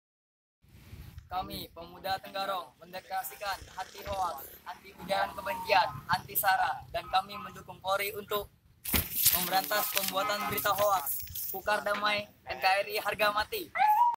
Deklarasi anti Hoax oleh Pemuda Tenggarong
Bertempat di Jl. K. H Akhmad Muksin Kel. Timbau Kec. Tenggarong Telah dilaksanakan Deklarasi anti HOAX dan Penyebaran Kebencian serta Dukungan Terhadap Polri dalam Penegakkan Hukum terhadap Pelaku HOAX oleh Pemuda Tenggarong Kab. Kutai Kartanegara